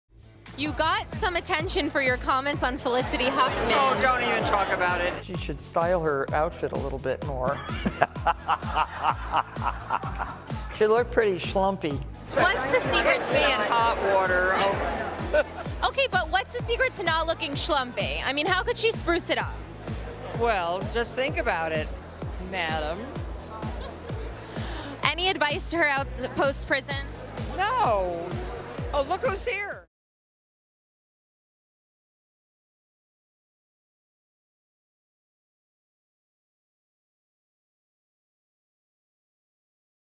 Martha Stewart Reacts To Her Comment On Felicity Huffman's Prison Look: 'It Gets Me In Hot Water'
Martha Stewart is zipping her lips when it comes to Felicity Huffman! At the premiere of "Very Ralph," the businesswoman expertly deflected questions about her recent comments about the "Desperate Housewives" alum's prison style. "Oh don't even talk about it, it gets me in hot water," she told Access Hollywood. Martha also declined to give post-prison advice to Felicity, who's sentence will be over later this month.